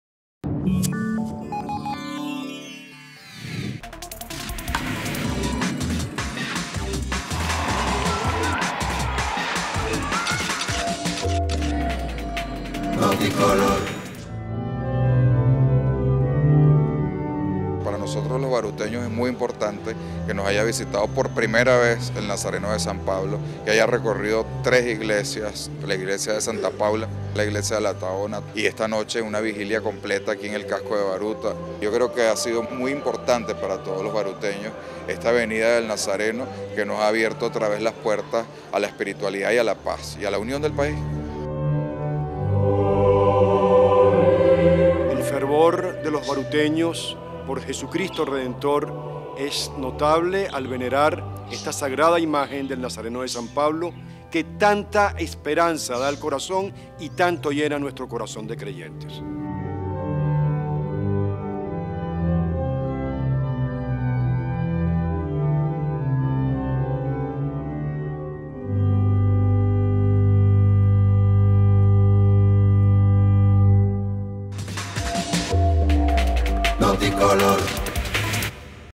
NOTICOLOR: Visita del Nazareno al Municipio Baruta.mov
Gerardo Blyde comenta sobre la visita del Nazareno a tres iglesias del Municipio Baruta: La Tahona, Santa Paula y la iglesia del pueblo de Baruta. Monseñor Fernando Castro habla también del fervor de los baruteños hacia el Nazareno de San Pablo, "que tanta esperanza da al corazón".